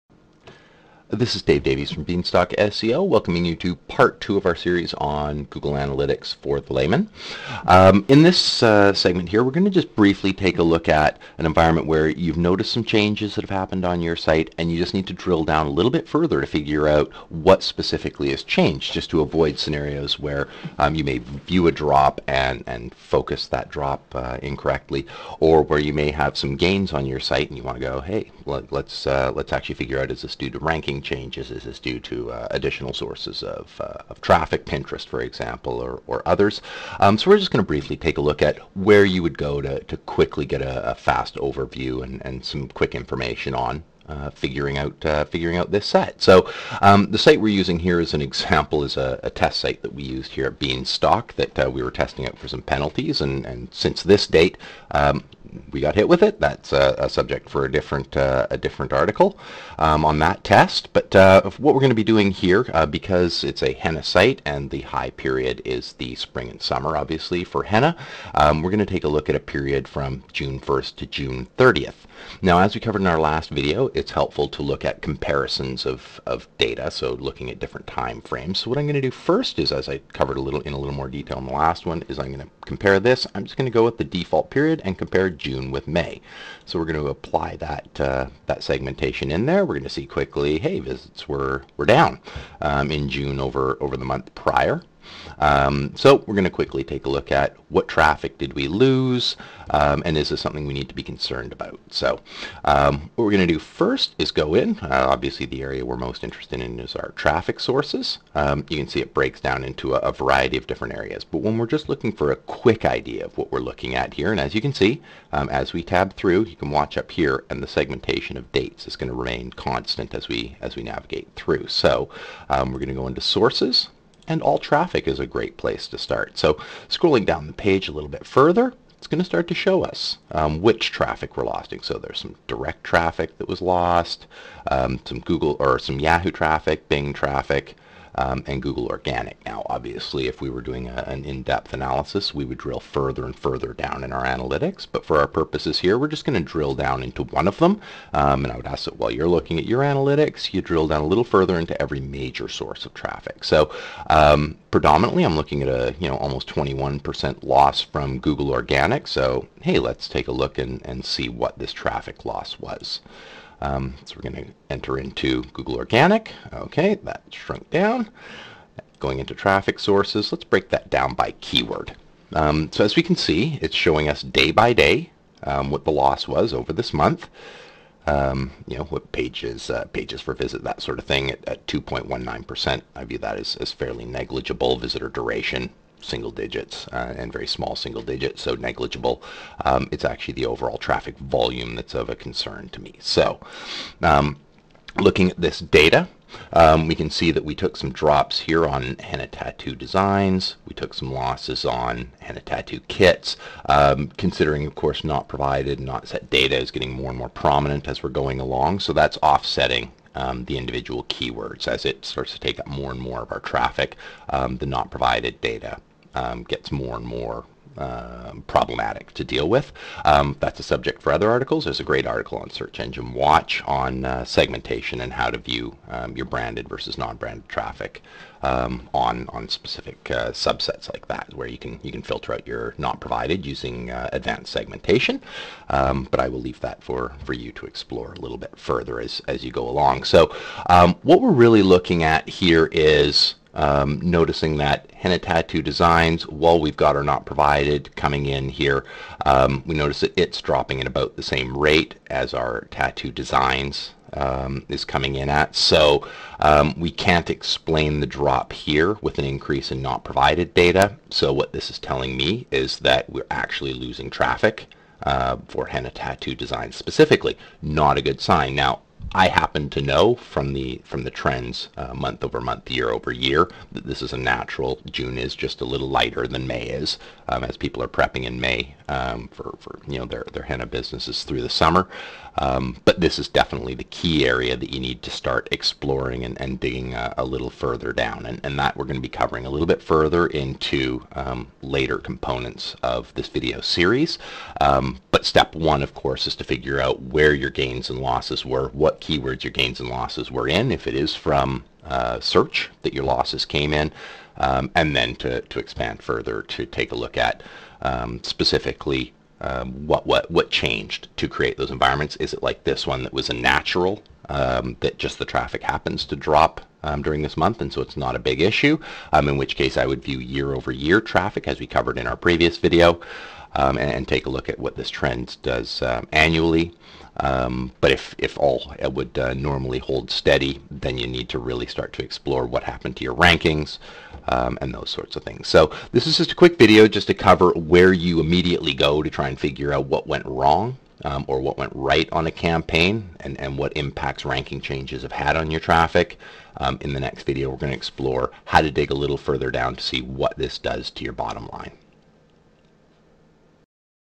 Finding Traffic Changes In Google Analytics
In this brief video we'll explore how to quickly drill down in your traffic to determine hwat